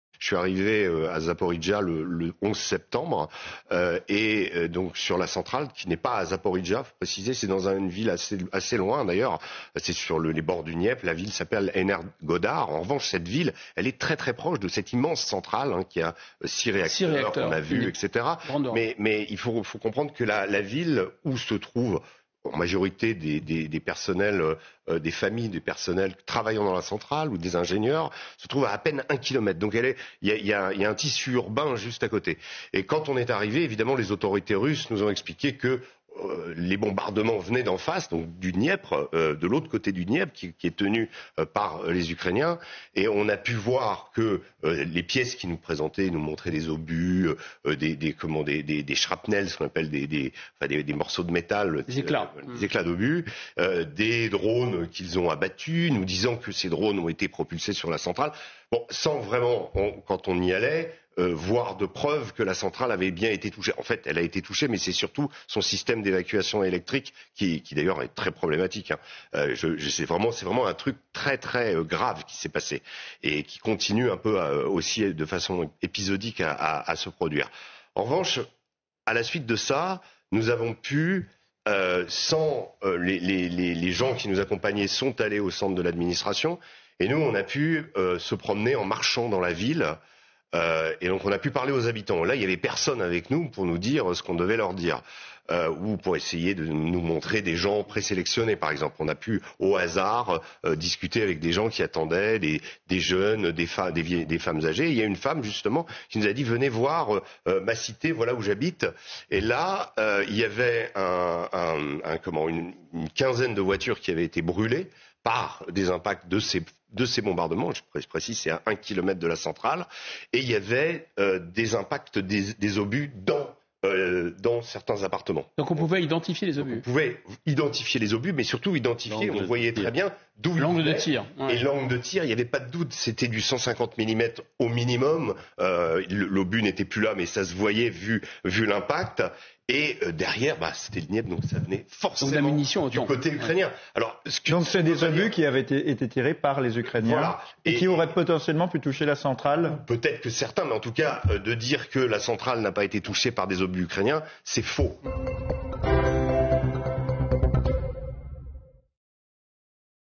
Régis le Sommier, confirme que l'Ukraine bombarde les immeubles civils et la central nucléaire !!
(Republication d'une vidéo publiée le 7 octobre 2022)